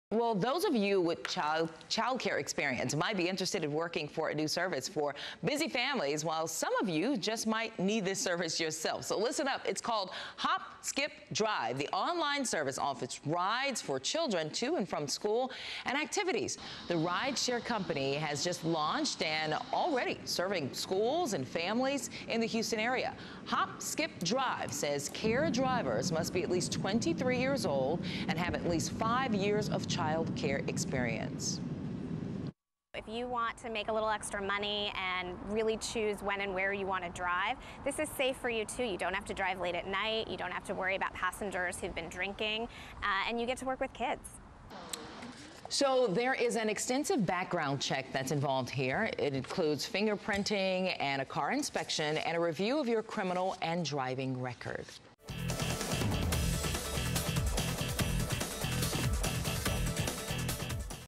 New rideshare service for kids is here
It's already working with more than 170 schools and districts nationwide in cities in Arizona; California; Colorado; and Virginia; as well as Washington, D.C.